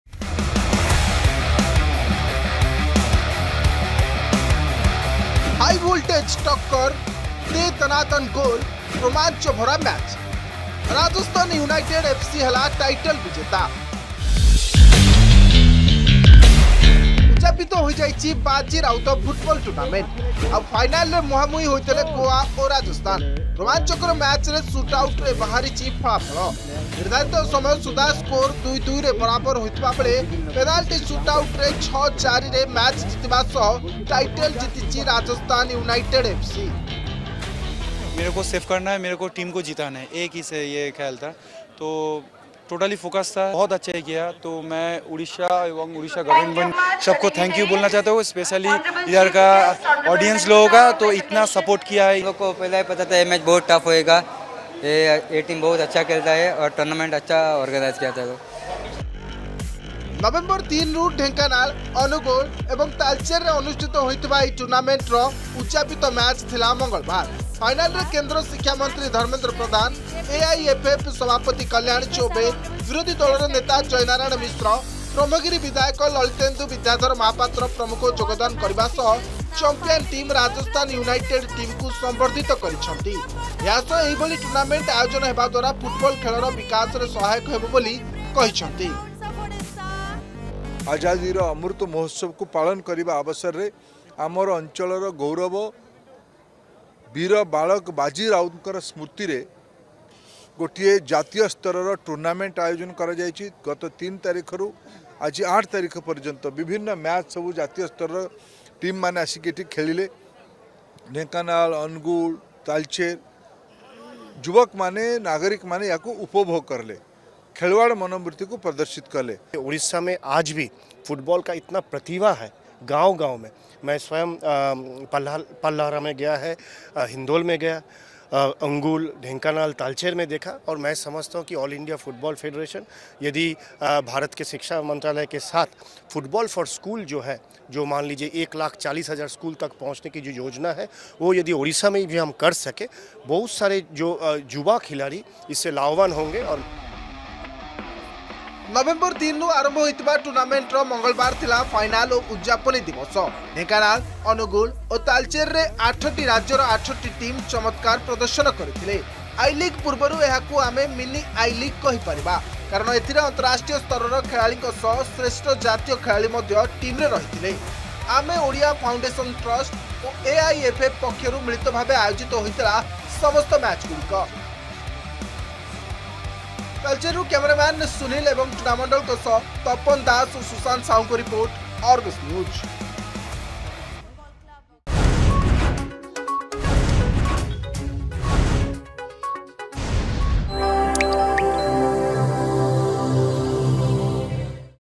ଉଦଯାପିତ ହେଲା ବାଜି ରାଉତ କପ ଫୁଟବଲ ଟୁର୍ନାମେଣ୍ଟ
ଉଦଯାପିତ ହୋଇଯାଇଛି ବାଜି ରାଉତ ଫୁଟବଲ ଟୁର୍ନାମେଣ୍ଟ । ଫାଇନାଲରେ ମୁହାଁମୁହିଁ ହୋଇଥିଲେ ଗୋଆ ଓ ରାଜସ୍ଥାନ
ରୋମାଞ୍ଚକର ମ୍ୟାଚରେ ସୁଟଆଉଟରେ ବାହାରିଛି ଫଳାଫଳ